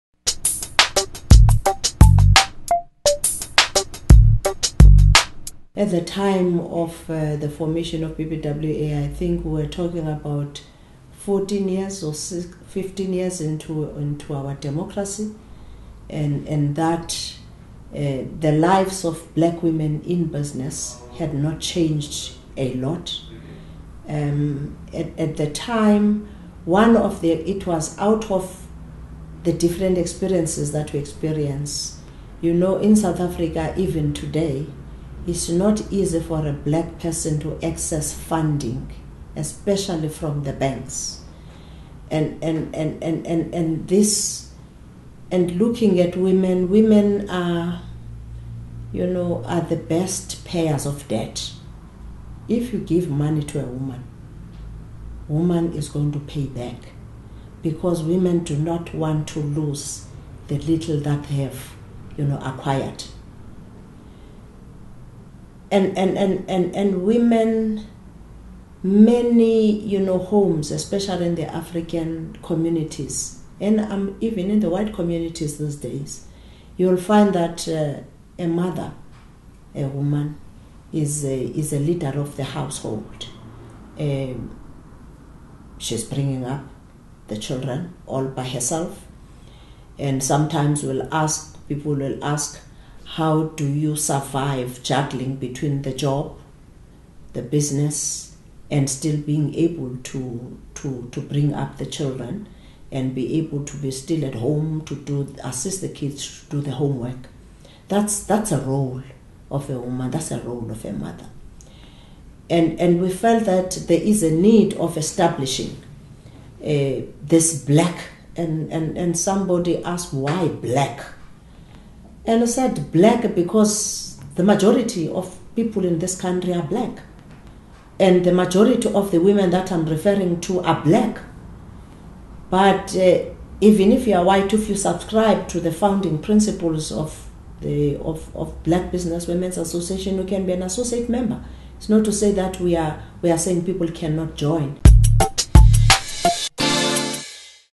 Black Business Women's Association: The formation of BBWA
The Black Business Women's Association (BBWA) chairperson, Hilda Ndude, talks about the formation of the organisation.